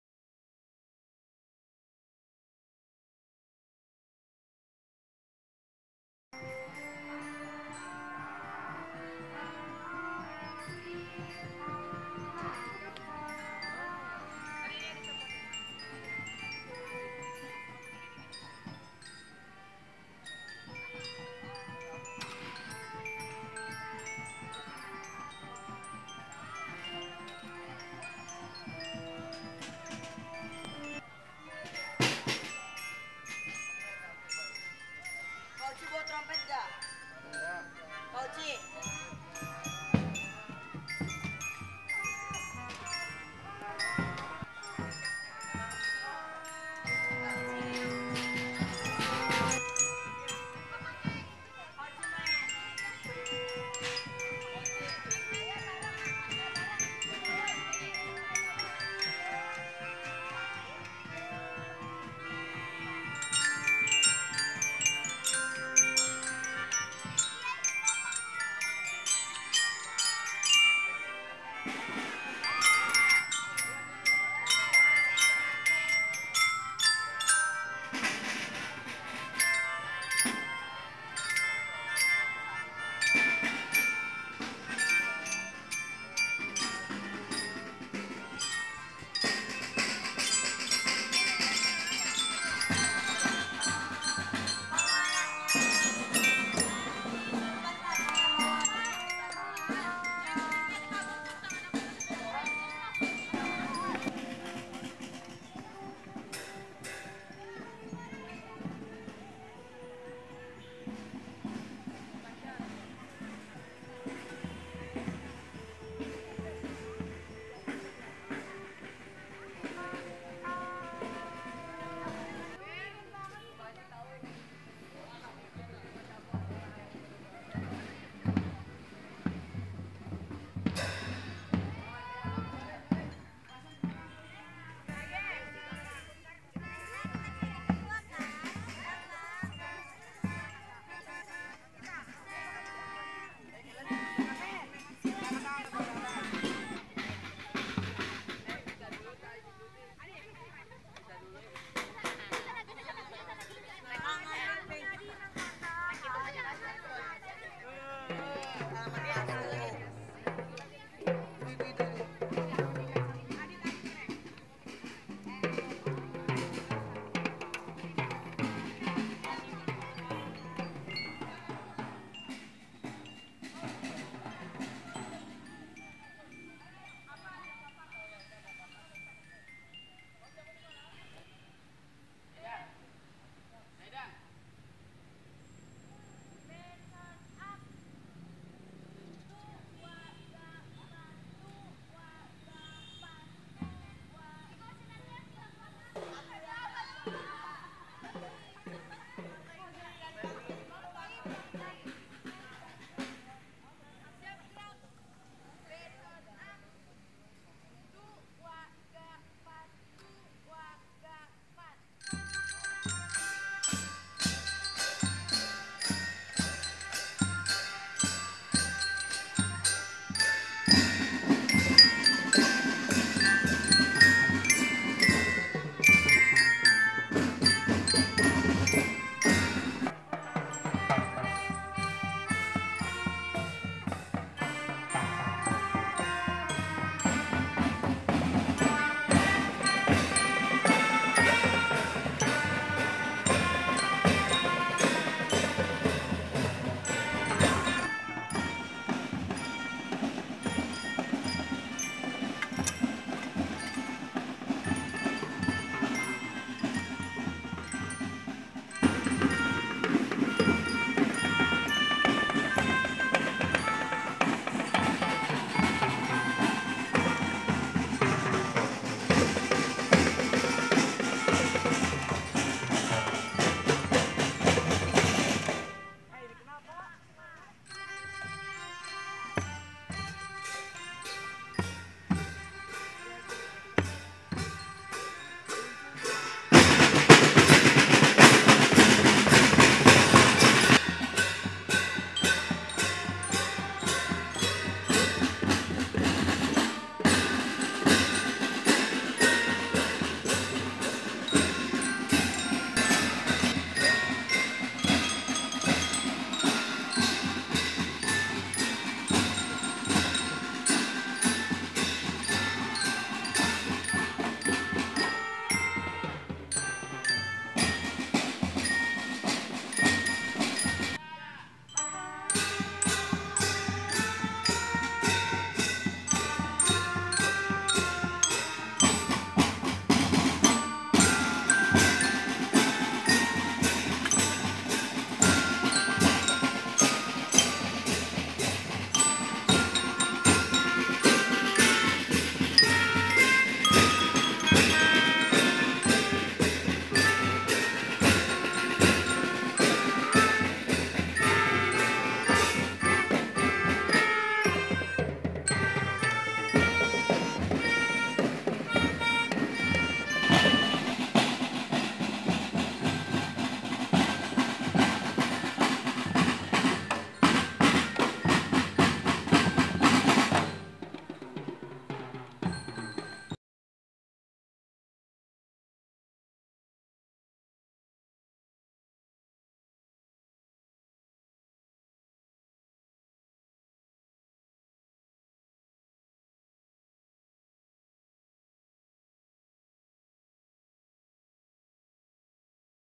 LATIHAN DRUM BAND
Anggota Ekstrakurikuler Marching Band SMPN 1 Kuningan sedang menjalani latihan yang di latih oleh Pak Uci Sanusi yang merupakan alumni Pondok Pesantren Al-Ikhlash.

Don't forget;

My Daily Video Channel